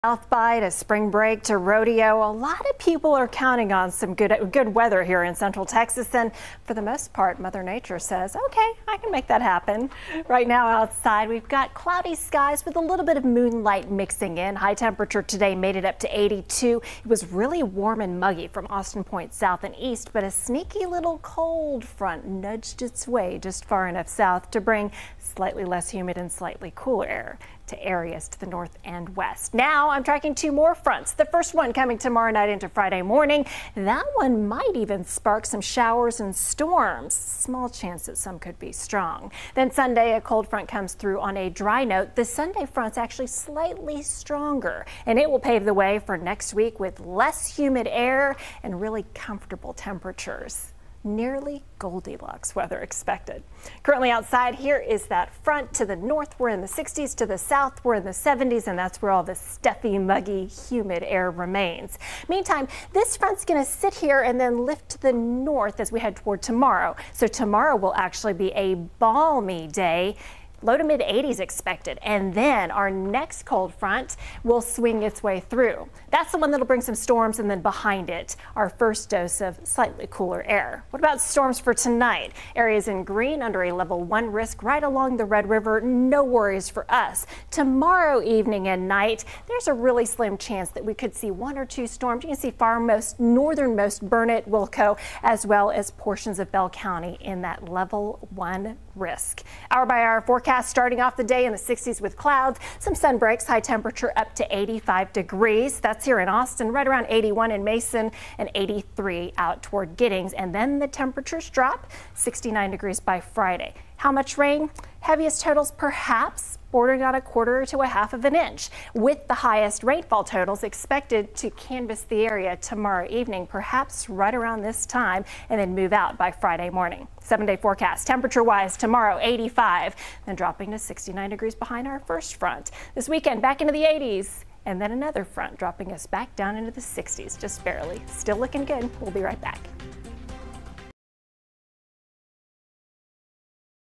Two cold fronts on the way - ChiksClips - Wednesday, March 8th, 2023
Temp swings and cold fronts, but no really cold air expected. Storm chances increasing for Thursday night to Friday morning.
ICYMI: ChiksClips - Wednesday, March 8th, 2023